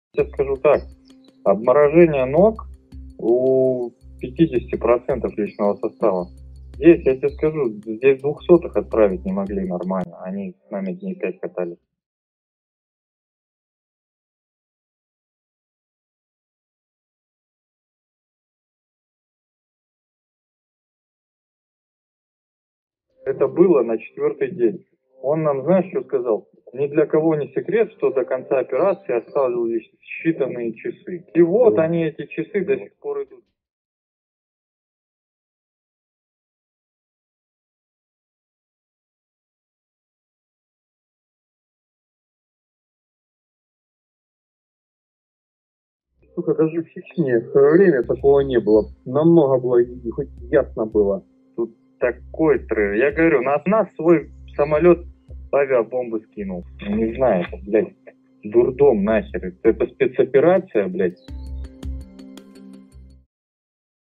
Cargo 200 is Russian military slang for soldiers that are killed in action
It doesnt sound like they are happy, lol.